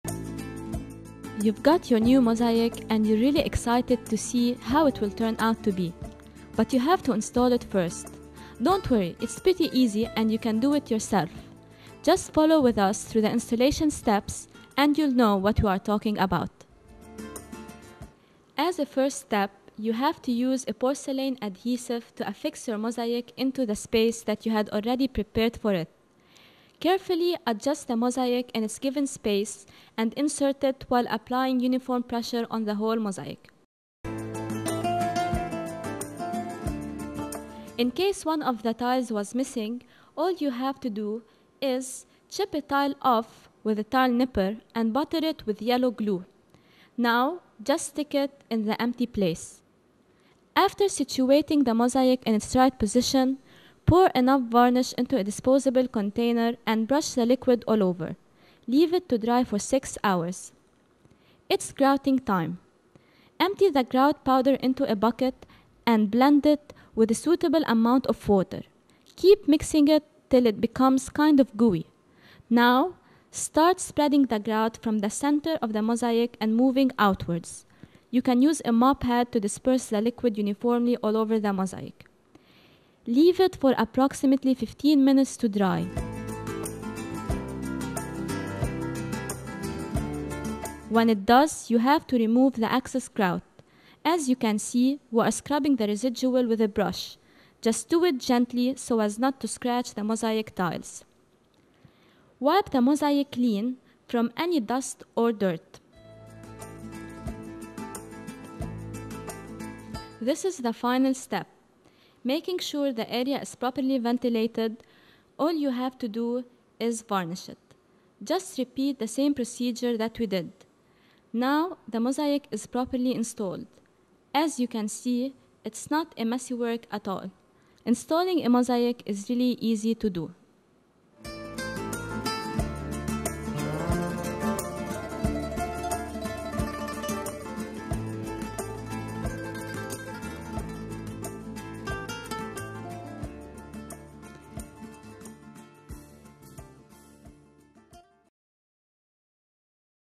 Tutorial On How To Install Your Mosaic Art
Installing a mosaic art for the first time can be difficult. In this video, you can find all the steps and materials to easily install your mosaic.